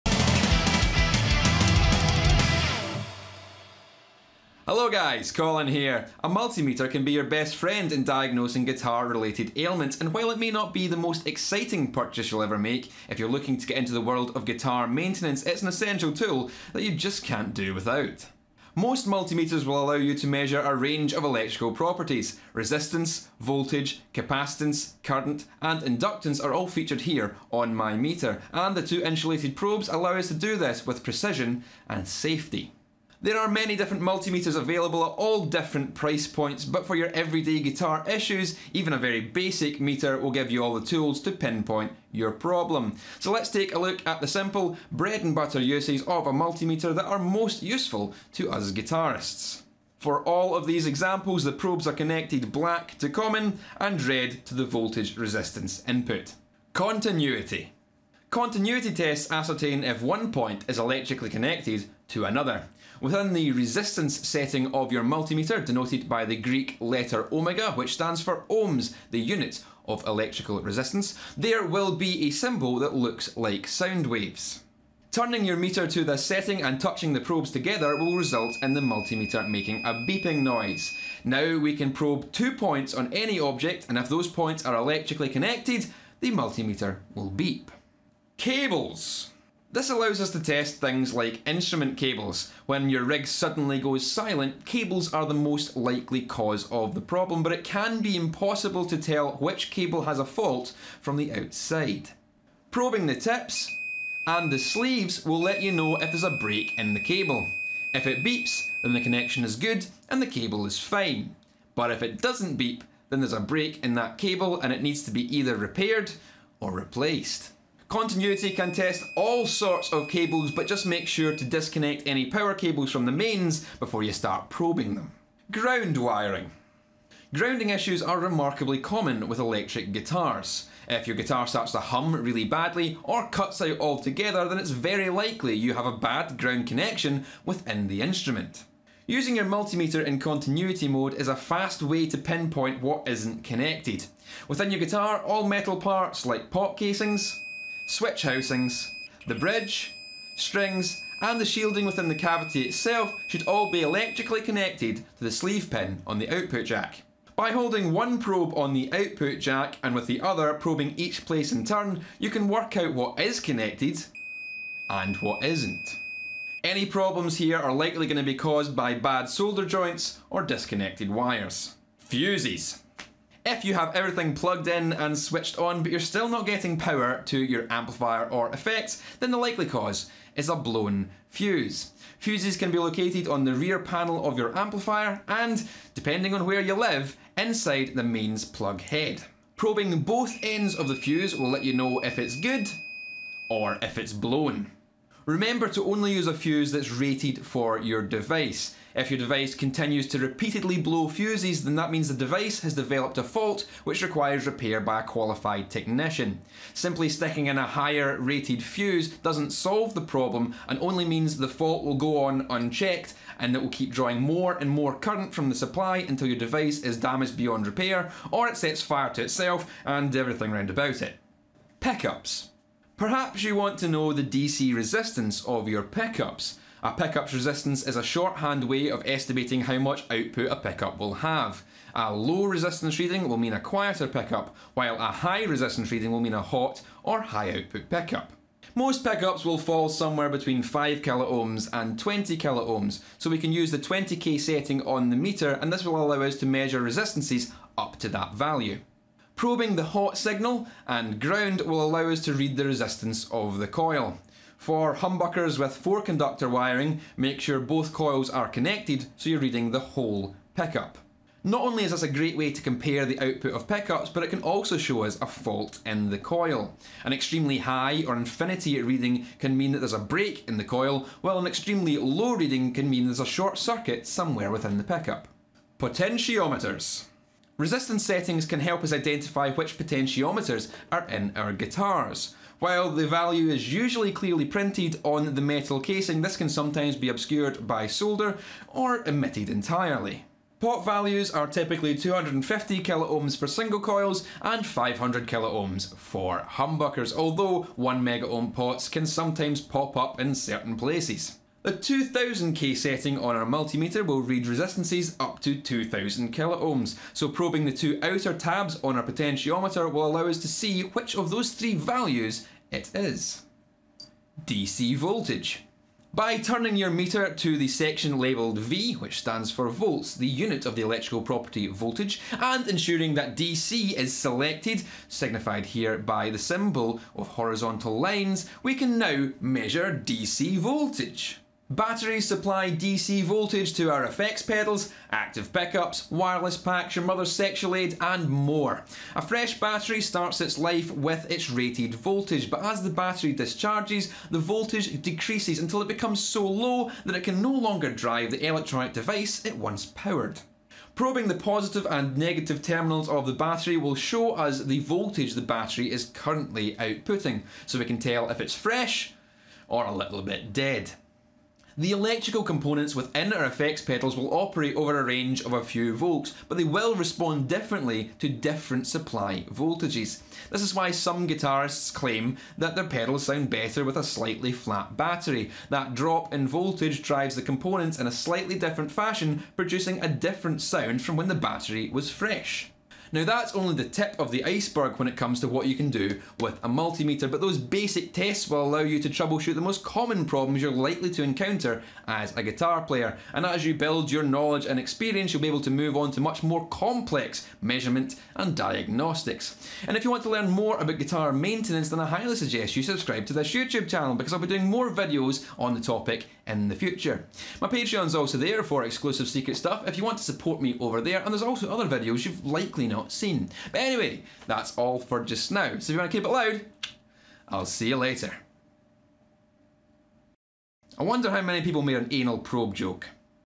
Multimeter: Guitar Diagnostics
A mulitmeter can be a useful tool for diagnosing many guitar problems. This video covers the basic uses of a multimeter to equip you to troubleshoot the most common guitar related ailments.

CSGuitars uses:
Dragon's Heart Guitar Picks: